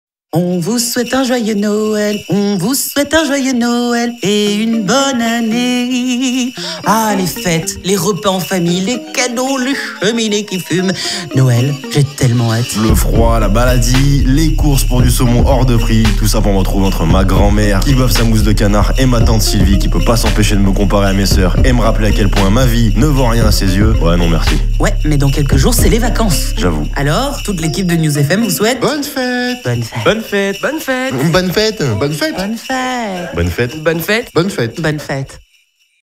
Bonnes Fêtes New'sFm
New'sFm - Studio : New'sFm - Date de diffusion : Decembre - Campagne diffusée sur New'sFm

Jingle radio réalisé entièrement par l’équipe de la Radio New'sFm. Ecriture, mise en forme, enregistrement, mixage, mastering.
Voix principale : Leanna et Eric (animateurs)
Voix bonne fêtes : Farid (Directeur), Magalie (Secrétaire), Gregory (Presse), Leanna et Eric (Animateurs)